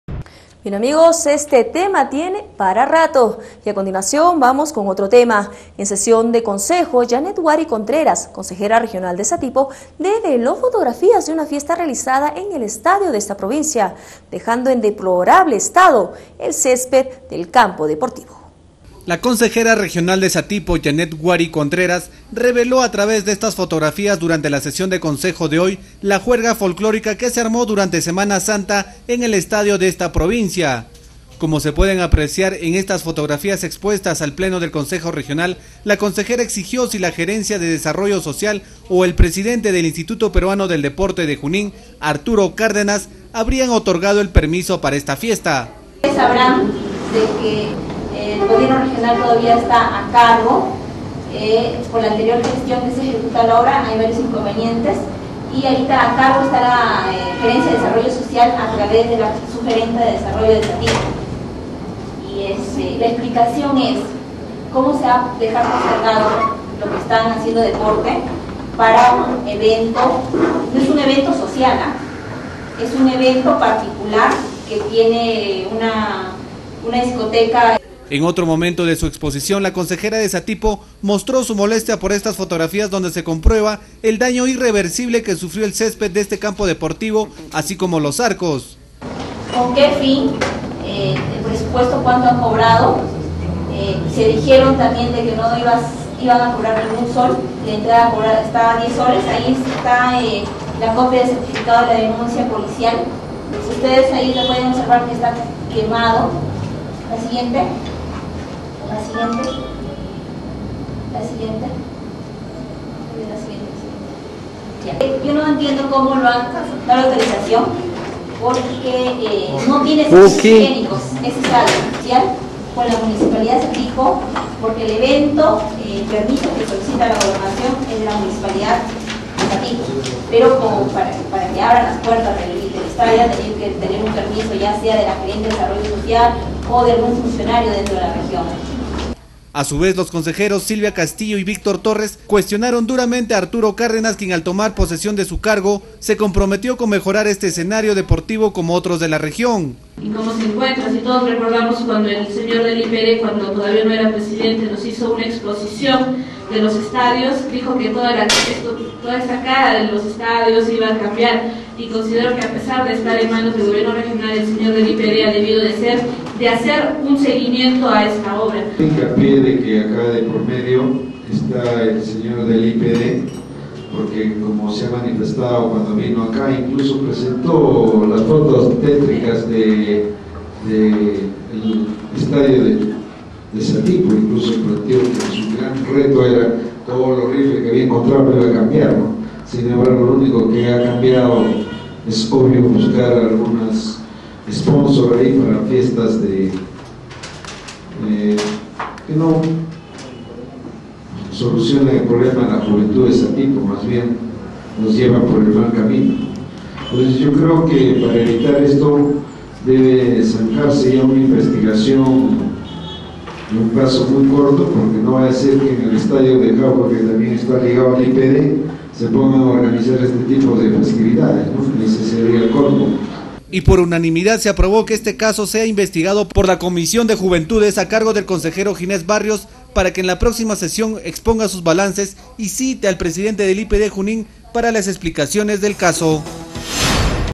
UTILIZAN ESTADIO PARA JUERGA SANTA.
En sesión de Consejo, Yaneth Huari Contreras, consejera regional de Satipo develo fotografías de una fiesta realizada en el estadio de esta provincia, dejando en deplorable estado en césped de este campo deportivo.